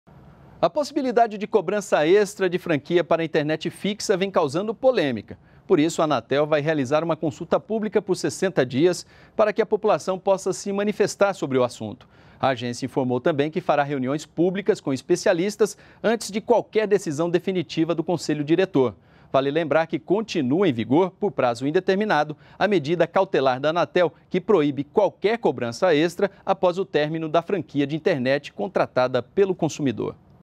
Anatel vai realizar consulta pública sobre franquia de internet
JORNAL NBR - 08.06.16: A possibilidade de cobrança extra pra franquia de internet fixa vem causando polêmica. Por isso, a Anatel vai abrir consulta pública por 60 dias para que a população possa se manifestar sobre o assunto.